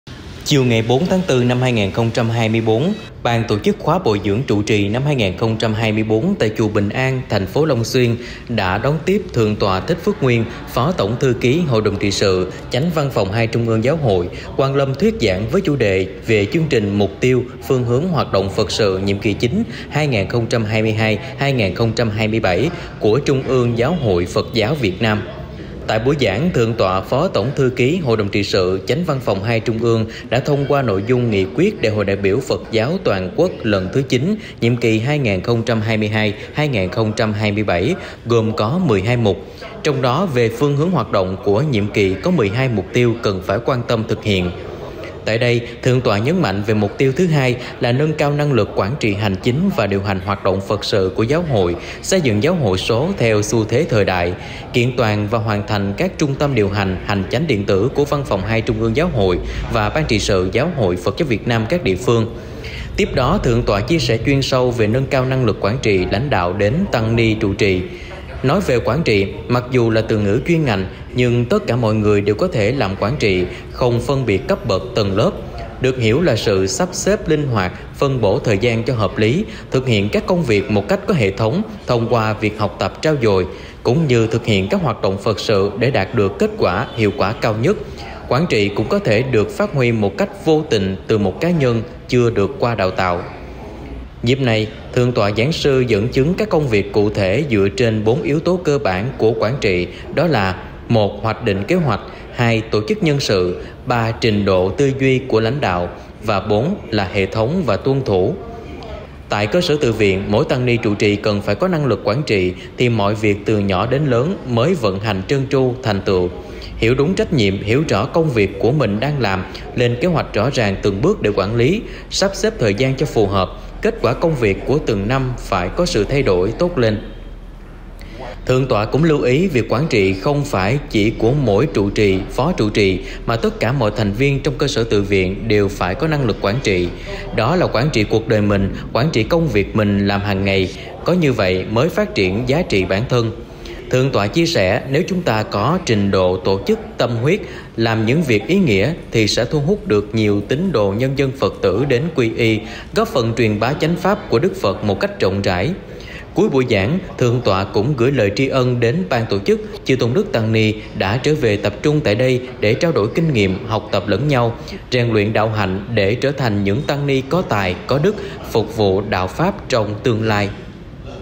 TT. THÍCH PHƯỚC NGUYÊN GIỚI THIỆU VỀ CHƯƠNG TRÌNH MỤC TIÊU, PHƯƠNG HƯỚNG HOẠT ĐỘNG CỦA TƯ GHPGVN
AN GIANG: THƯỢNG TỌA THÍCH PHƯỚC NGUYÊN GIỚI THIỆU VỀ CHƯƠNG TRÌNH MỤC TIÊU, PHƯƠNG HƯỚNG HOẠT ĐỘNG NHIỆM KỲ IX (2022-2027) CỦA TRUNG ƯƠNG GHPGVN